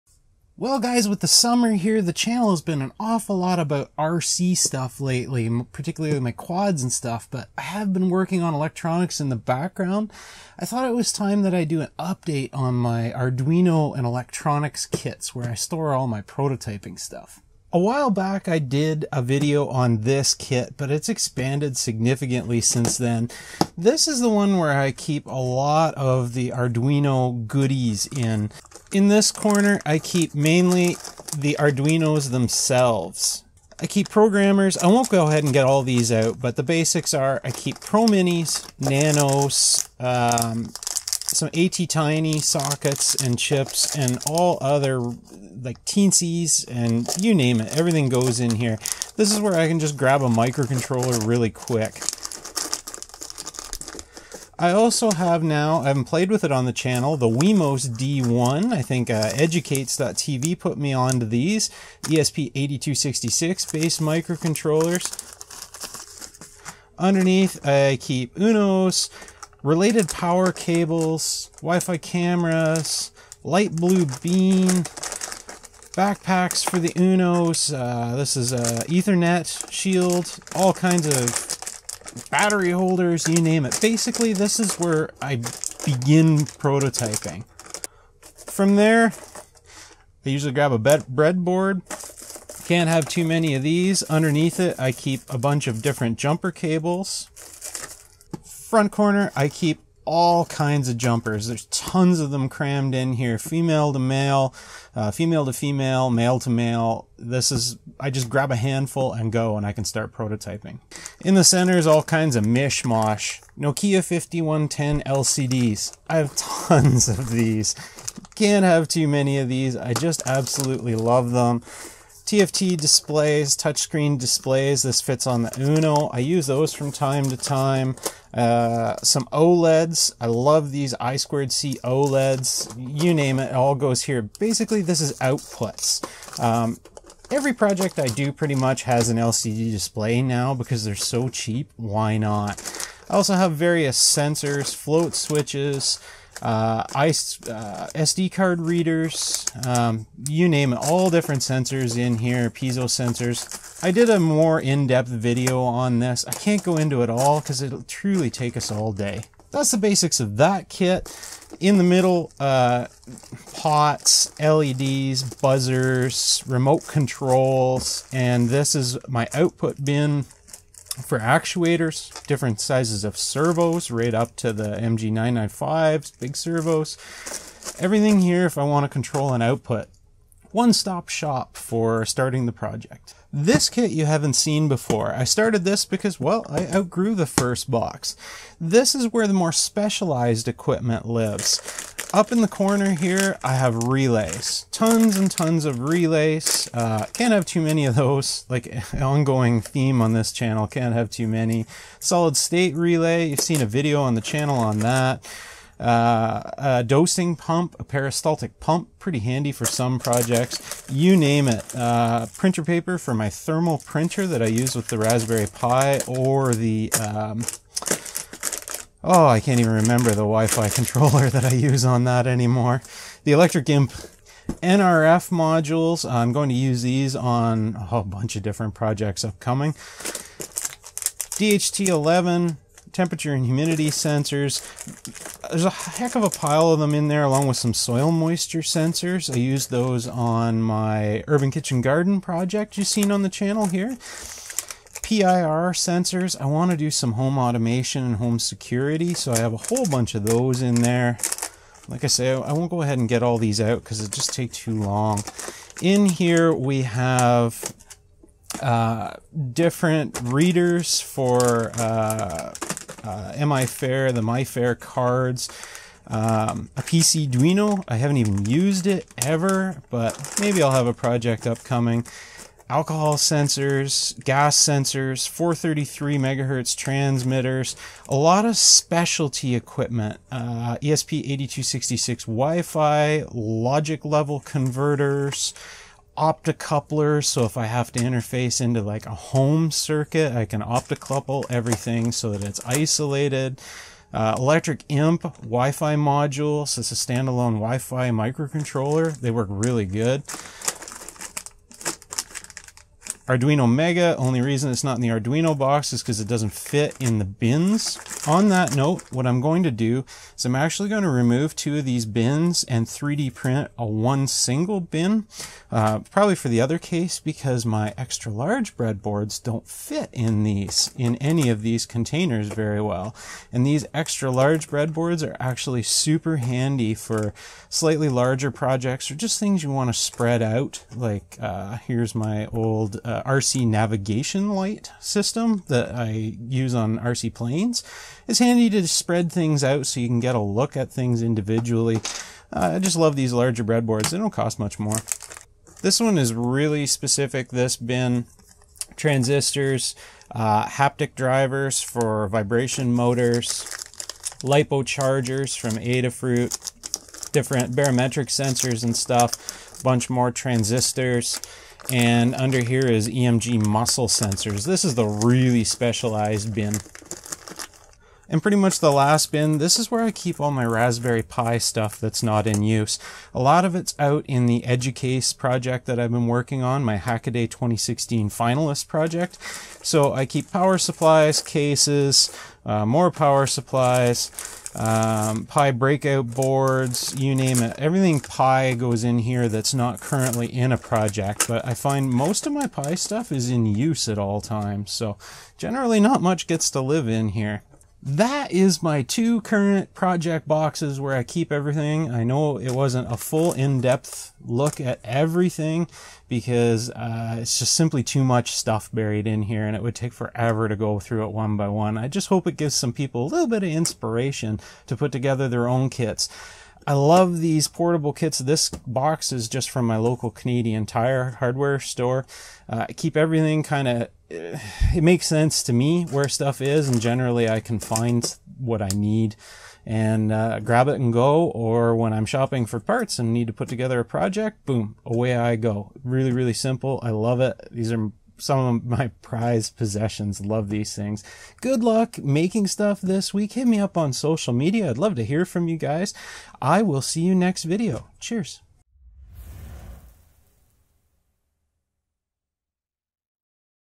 DIY Electronics & Arduino Kits For Making Projects & Rapid Prototyping
Kits containing all my electronics and Arduino parts I use most often in my DIY & Maker style projects & prototypes.

Way too many electronics and Arduino parts and sensors to list them all.  I grab the components from these kits any time I'm creating a new design or project.  Quickly I can get a project on the breadboard and the Arduino programmed.

I hope this inspires others to create their own electronics project boxes.  Having everything on hand (cheap parts from eBay) makes it far easier to jump right into any idea/project while you are still inspired.